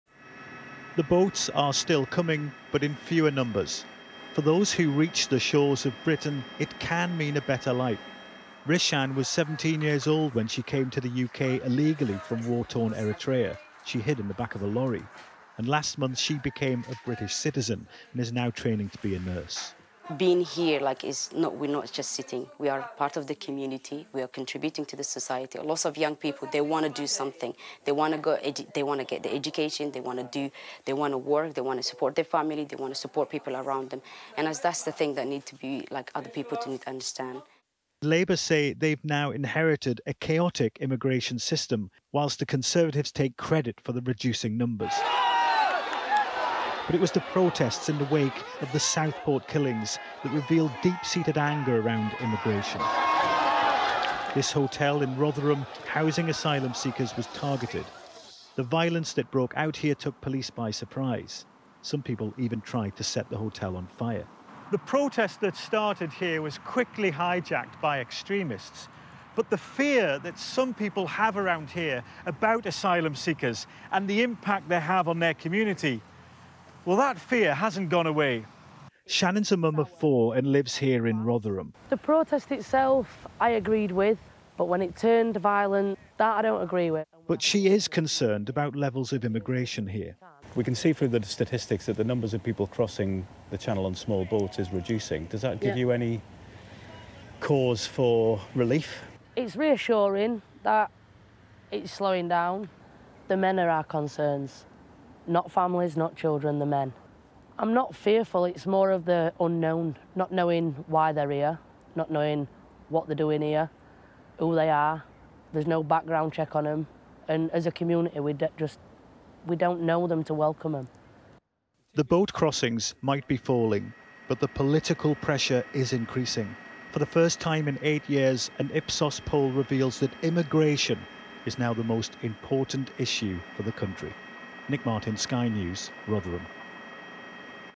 Migration: Significant drop in number of people coming from abroad
There's been a significant drop in the number of people coming from abroad to work or study in the UK.

Home Office data shows 31,493 people arrived by small boats in the 12 months to June 2024. That's 29% fewer than the 44,460 who made crossings by June 2023.